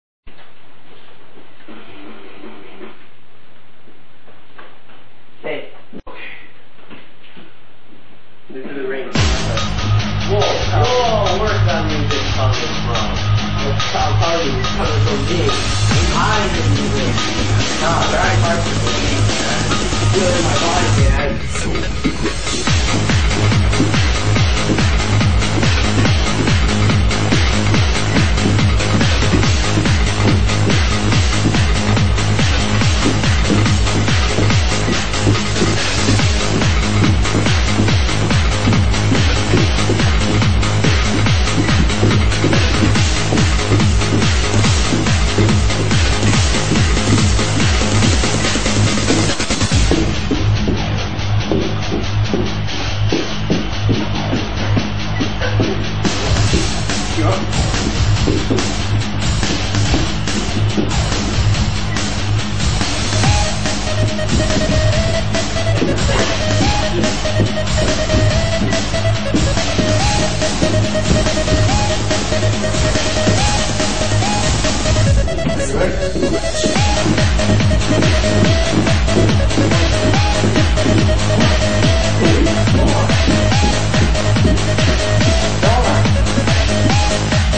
Cabaret Practice Fail (JB Tyrrell 2010)
I wasn't wearing pants and I can't freestyle so I failed when it was my solo.. CHEK IT OUT